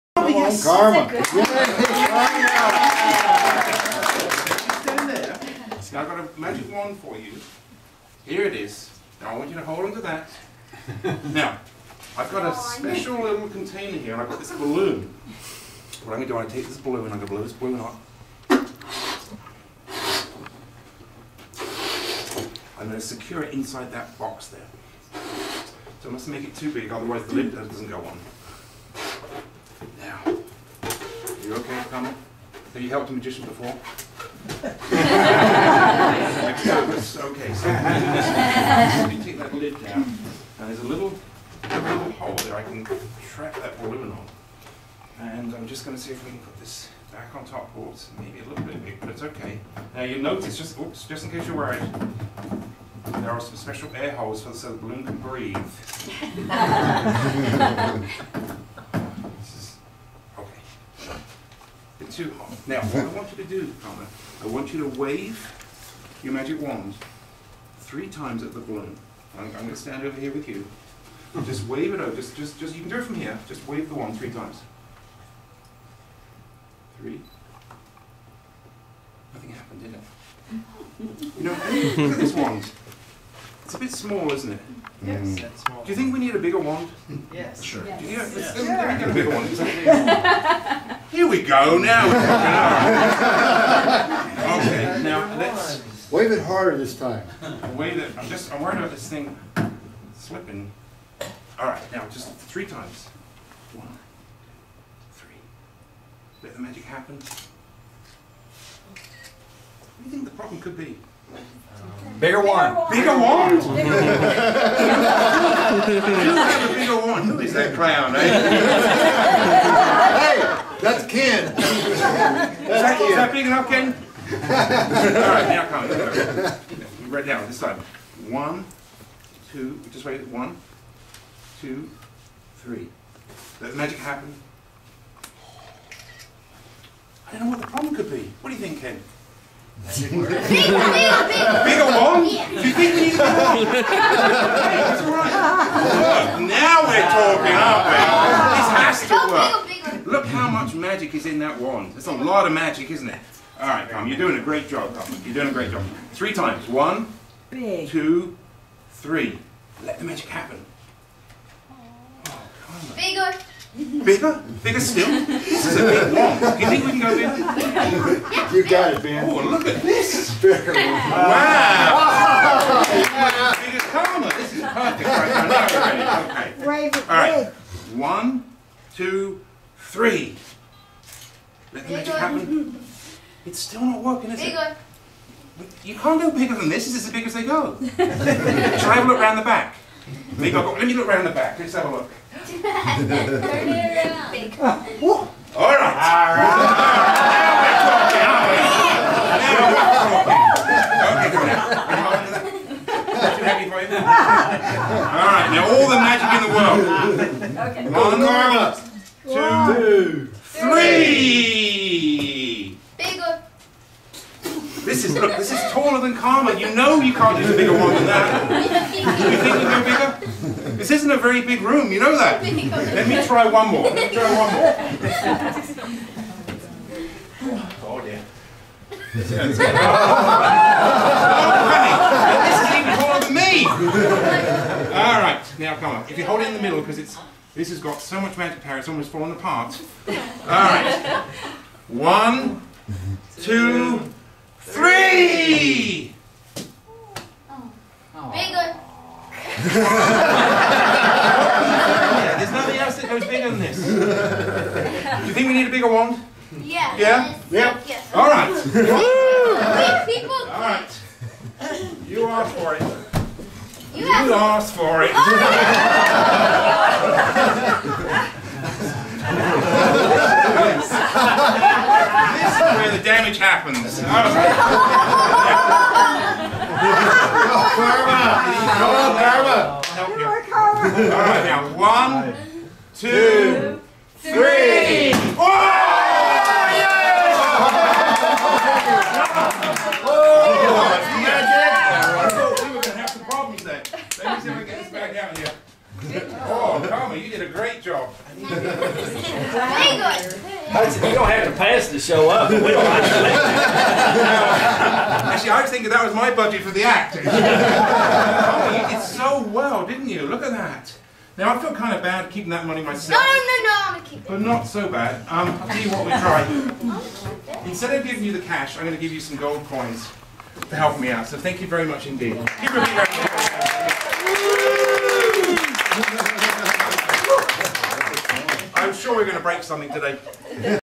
Creative Magic Bigger Wands (2012 Hudson)
This is my version of the wonderfully entertaining Bigger and Biggest Wands from Creative Magic.